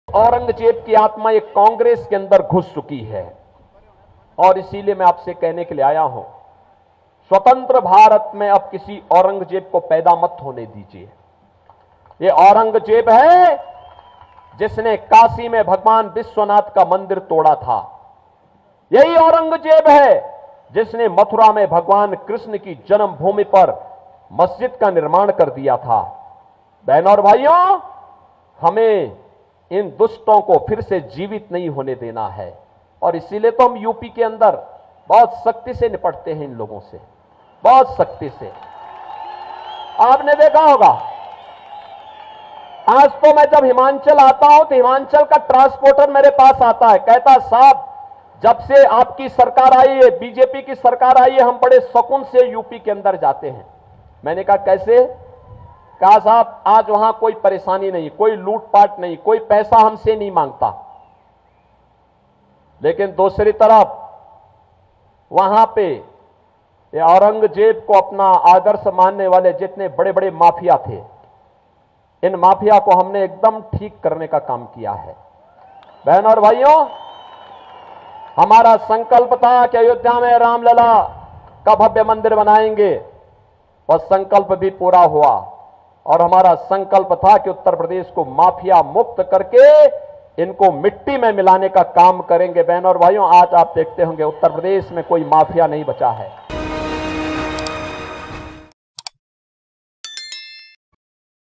CM Yogi in Himachal Pradesh: 'स्वतंत्र भारत में अब किसी Aurangzeb को पैदा मत होने दीजिए' - CM योगी
Yogi in Himachal Pradesh: 'स्वतंत्र भारत में अब किसी औरंगजेब को पैदा मत होने दीजिए' - CM योगी